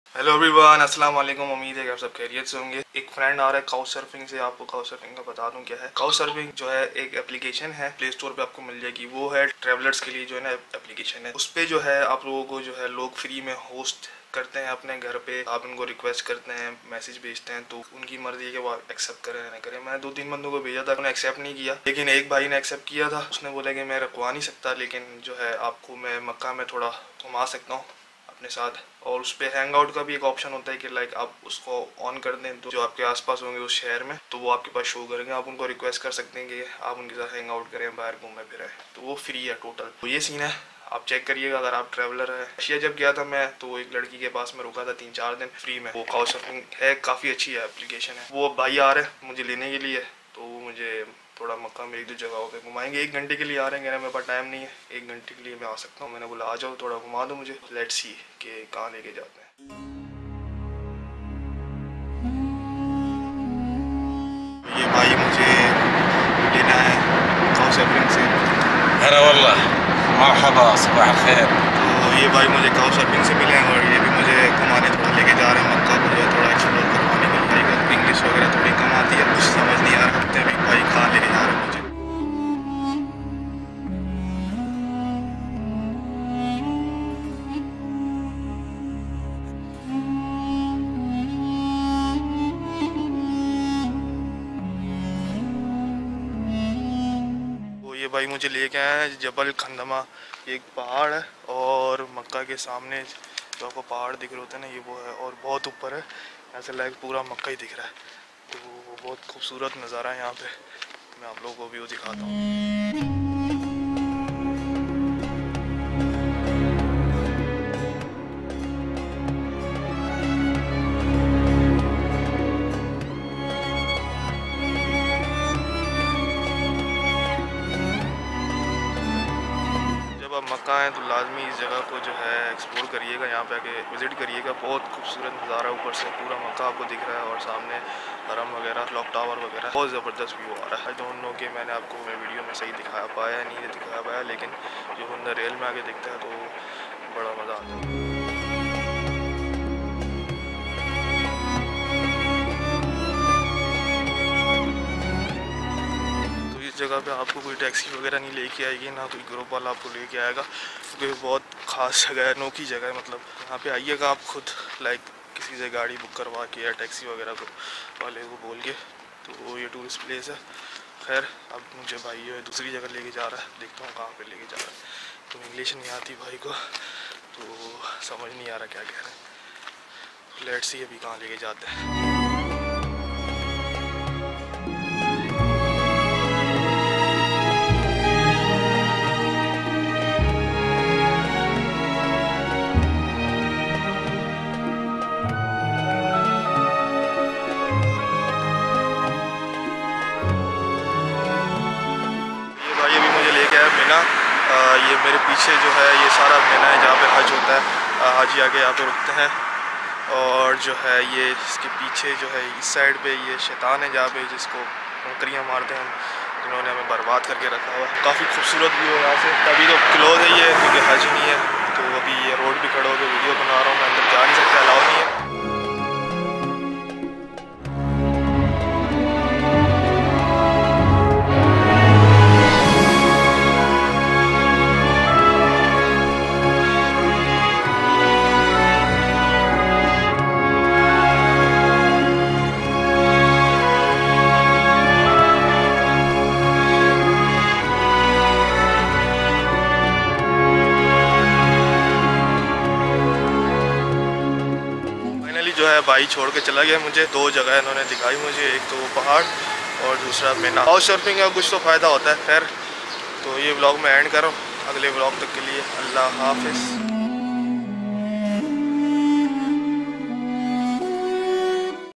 Explore Makkah Tourist Place | Hangout With COUCHSURFING Friend in Mecca
Hello Everyone Assalam O Alaikum | Is video mai COUCHSURFING k baray mai bataya hai mai ny. Makkah mai Hangout kea mai ny Couchsurfing Friend k sth. Woh mjhy 2 jaga lai kr gaya 1st Jabal Khandama or 2nd Mina. Bohat Khubsurat places the.

Pura Makkah Dikha Mujhay Yahan Sai | Hangout With COUCHSURFING Friend in Makkah